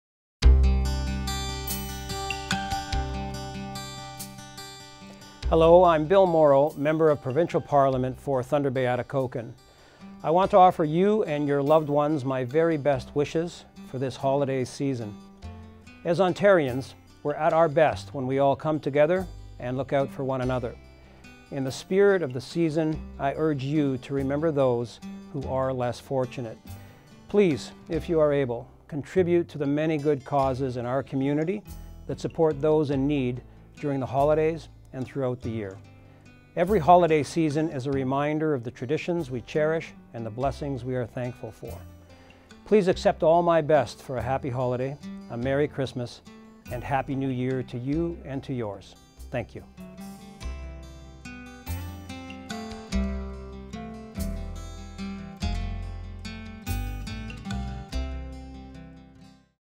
Bill Mauro Holiday Message 2016
A holiday greeting from Hon. Bill Mauro, Member of Provincial Parliament for Thunder Bay-Atikokan.